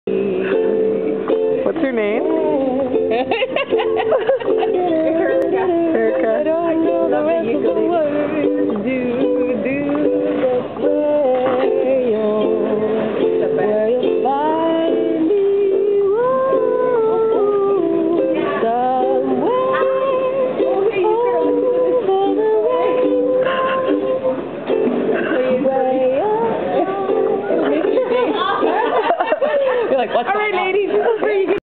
Babes For Breasts - Ukulele in the Airport
Ana Miura and Amanda Rheaume arrive in Vancouver for the first Babes for Breasts show of 2008!